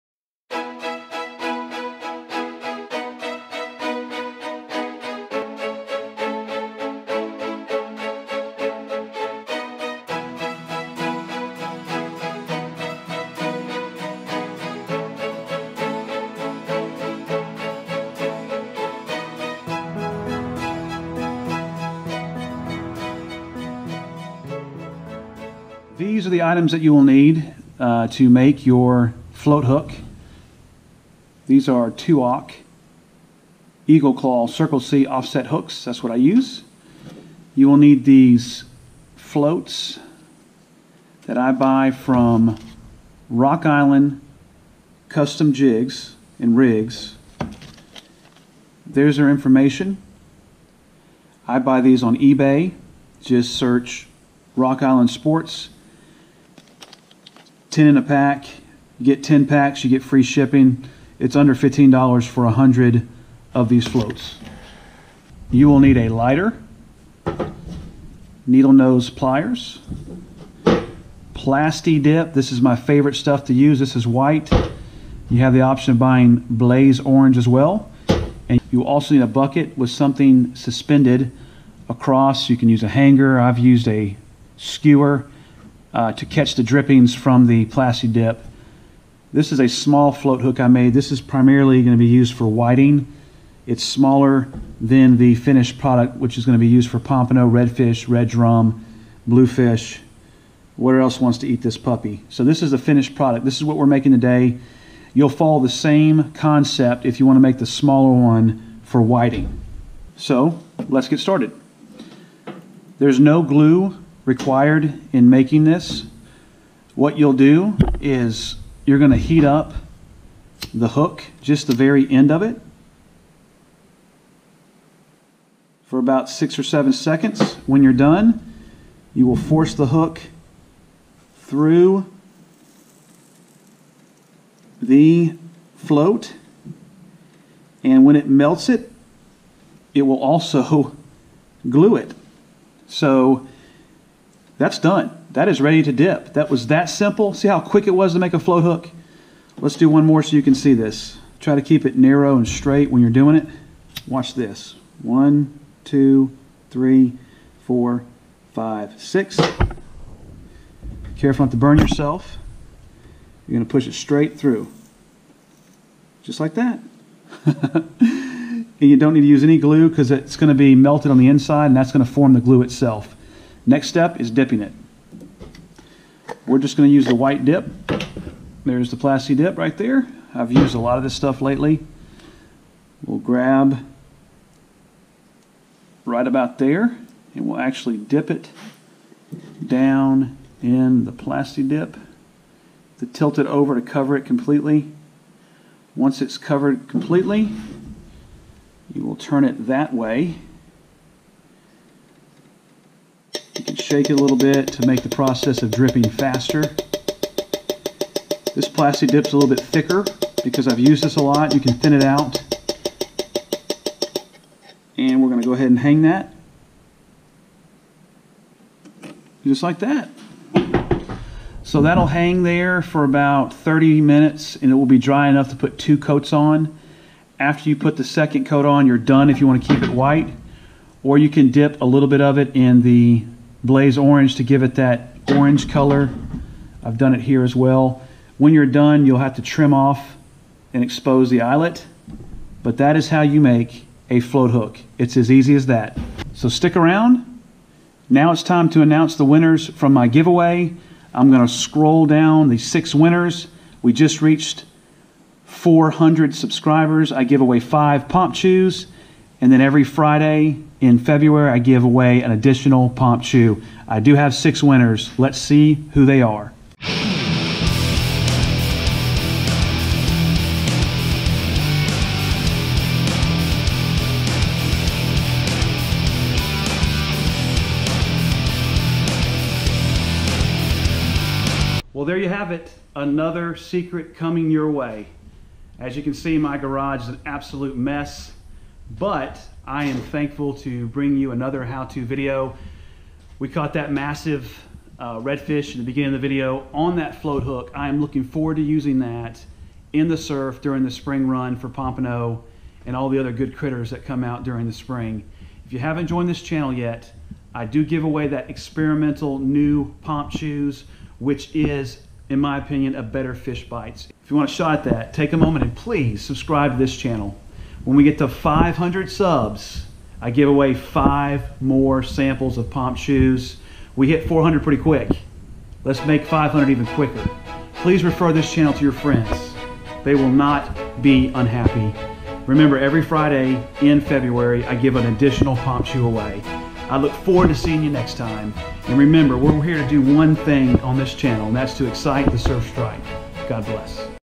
THE SECRET IS OUT FOR SURF FISHING! The SIMPLE FLOAT HOOK.
Here it is, another Secret Revealed to my Viewers! I hope it helps. You'll be making a SUPER Easy Float Hook! This works for Surf Fishing!